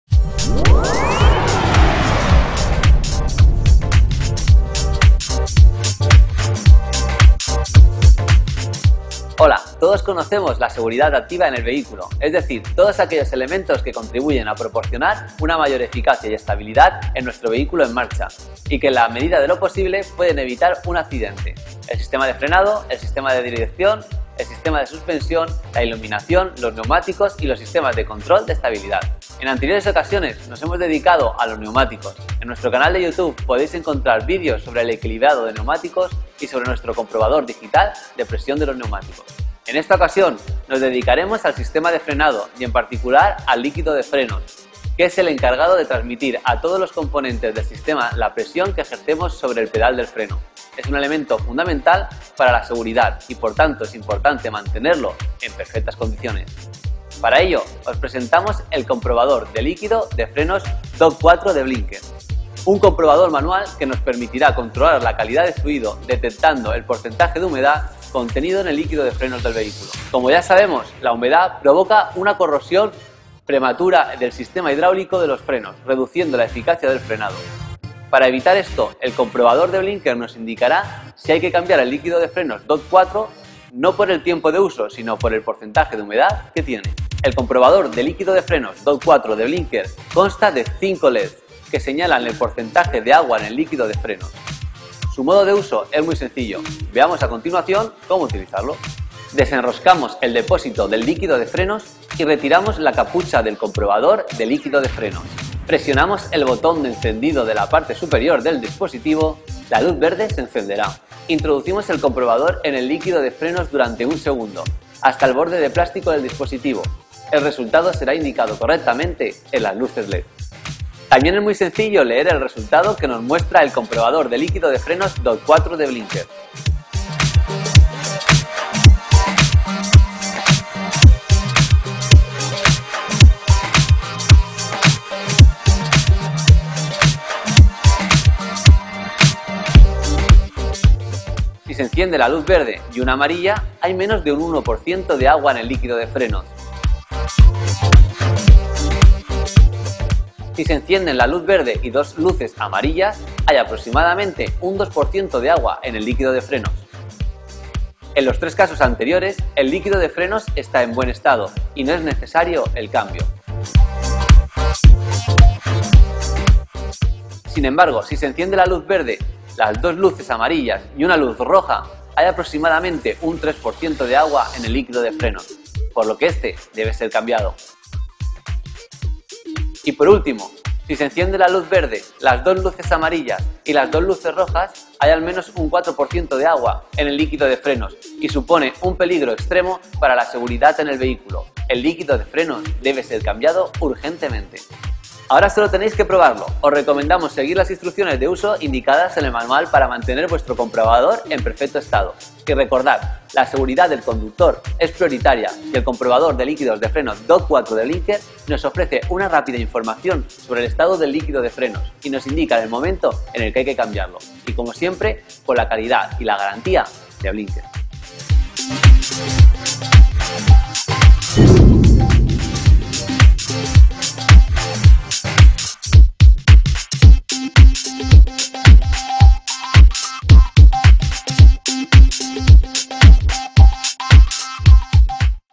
¿CUÁNDO DEBO CAMBIAR EL LÍQUIDO DE FRENOS?
Todos conocemos la seguridad activa en el vehículo, es decir, todos aquellos elementos que contribuyen a proporcionar una mayor eficacia y estabilidad a nuestro vehículo en marcha y que, en la medida de lo posible, pueden evitar un accidente: el sistema de frenado, el sistema de dirección, el sistema de suspensión, la iluminación, los neumáticos y los sistemas de control de estabilidad.

0:00 - ¿Cuando cambiar el líquido de frenos?
0:09 - Inicio del vídeo
0:57 - Presentación del comprobador de líquido de frenos DOT4 Blinker
1:35 - Modo de uso: averigua cuando cambiar el líquido de frenos.
2:00 - Funcionamiento del DOT4 Blinker

En anteriores ocasiones nos hemos dedicado a los neumáticos; en nuestro canal de YouTube podéis encontrar vídeos sobre el equilibrado de neumáticos y sobre nuestro comprobador digital de presión de los neumáticos.

En esta ocasión nos dedicaremos al sistema de frenado y, en particular, al líquido de frenos, que es el encargado de transmitir a todos los componentes del sistema la presión que ejercemos sobre el pedal de freno. Es un elemento fundamental para la seguridad y, por tanto, es importante mantenerlo en perfectas condiciones.

Para ello os presentamos el comprobador de líquido de frenos DOT4 de Blinker, un comprobador manual que nos permitirá controlar la calidad del fluido, detectando el porcentaje de humedad contenido en el líquido de frenos del vehículo.
Como ya sabemos, la humedad provoca una corrosión prematura del sistema hidráulico de los frenos, reduciendo la eficacia de frenada; para evitar esto, el comprobador de Blinker nos indicará si hay que cambiar el líquido de frenos DOT4, no por el tiempo de uso sino por el porcentaje de humedad que tiene.

El comprobador de líquido de frenos DOT4 de Blinker consta de 5 LEDs que señalan el porcentaje de agua en el líquido de frenos.  Su modo de uso es muy sencillo: veamos a continuación cómo utilizarlo.

1. Desenroscamos el depósito del líquido de frenos y retiramos la capucha del comprobador de líquido de frenos.

2. Presionamos el botón de encendido de la parte superior del dispositivo; la luz verde se encenderá.

3. Introducimos el comprobador en el líquido de frenos durante un segundo hasta el borde de plástico del dispositivo; el resultado será indicado correctamente en las luces LED.

También es muy sencillo leer el resultado que nos muestra el comprobador de líquido de frenos DOT4 de Blinker:
• si se enciende la luz verde, no hay agua contenida en el líquido de frenos.
• si se encienden la luz verde y una amarilla, hay menos de un 1% de agua en el líquido de frenos.
• si se encienden la luz verde y dos luces amarillas, hay aproximadamente un 2% de agua en el líquido de frenos.

En los 3 casos anteriores, el liquido de frenos está en buen estado y no es necesario el cambio.

• Sin embargo, si se encienden la luz verde, las dos luces amarillas y una luz roja, hay aproximadamente un 3% de agua en el líquido de frenos, por lo que éste debe ser cambiado.
• Y por último, si se encienden la luz verde, las dos luces amarillas y las dos luces rojas hay al menos un 4% de agua en el líquido de frenos y supone un peligro extremo para la seguridad en el vehículo: el líquido de frenos debe ser cambiado urgentemente.
Ahora solo tenéis que probarlo.

Os recomendamos seguir las instrucciones de uso indicadas en el manual para mantener vuestro comprobador en perfecto estado.

Y recordad: la seguridad del conductor es prioritaria y el comprobador de líquido de frenos DOT4 de Blinker nos ofrece una rápida información sobre el estado del líquido de frenos y nos indica el momento en el que hay que cambiarlo.

Y, como siempre, con la calidad y garantía de Blinker.

Si quieres adquirir este producto o tener más información, puedes contactarnos…
Por teléfono GRATUITO: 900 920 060

¿Te ha gustado? dale a "me gusta", comparte y/o déjanos un comentario..
Te esperamos en el próximo vídeo.